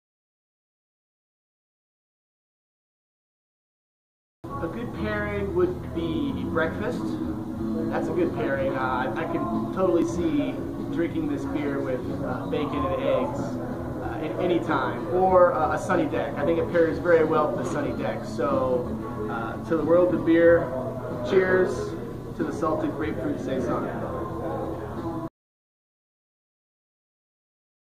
World Of Beer - Salted Grapefruit Farmhouse Ale Pairing
Pairing recommendations from Breckenridge Brewery for their Salted Grapefruit Farmhouse Ale | Tapping July 10th at your local WOB.